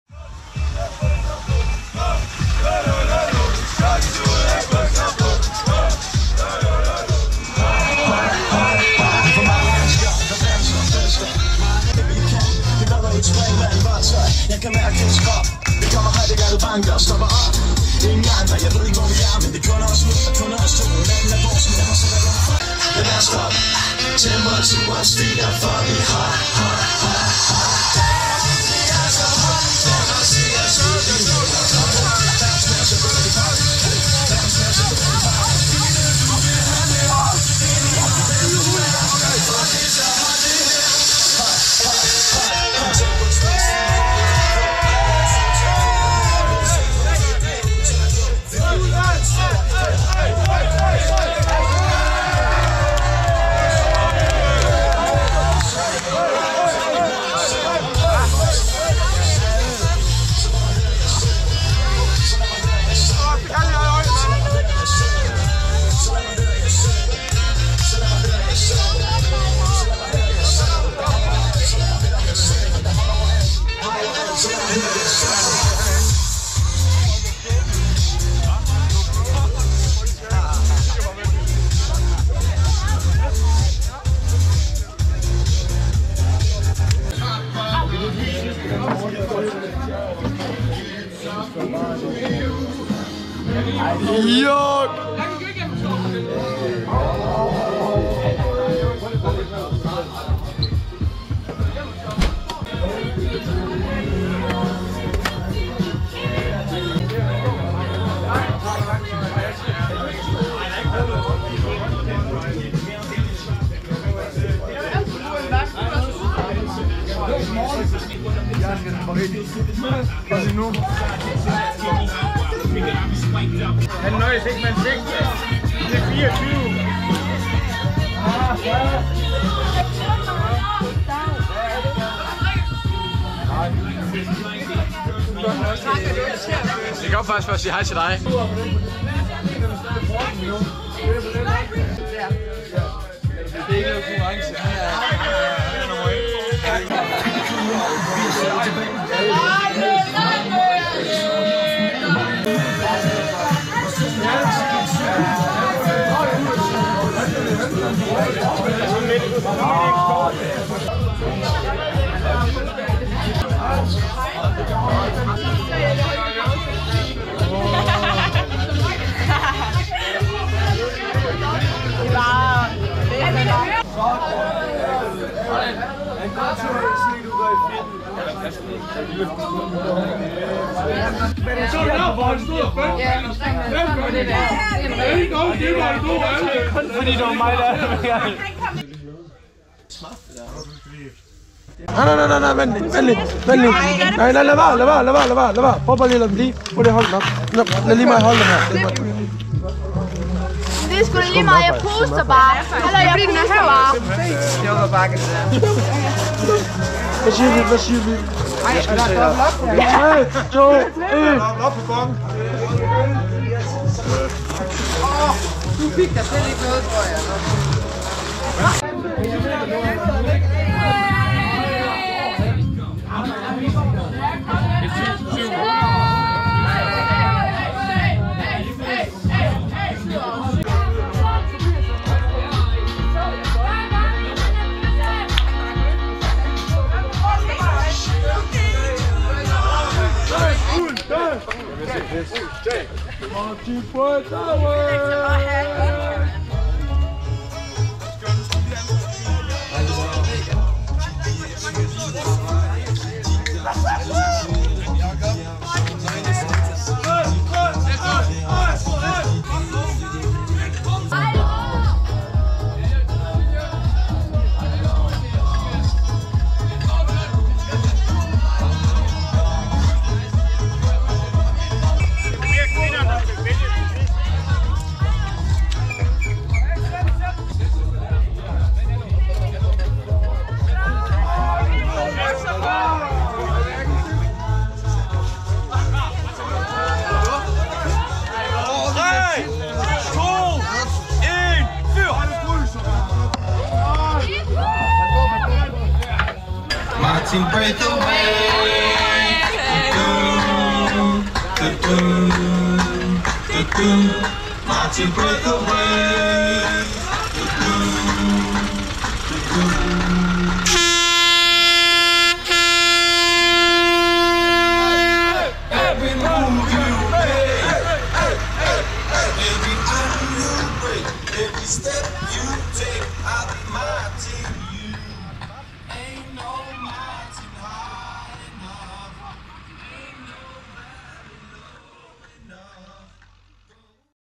Studenterkørsel 2021, Rybners HHX 3D
Ophold en times tid i Tjæreborg for optankning og så videre til næste optankning...Så kan livet da ikke være skønnere.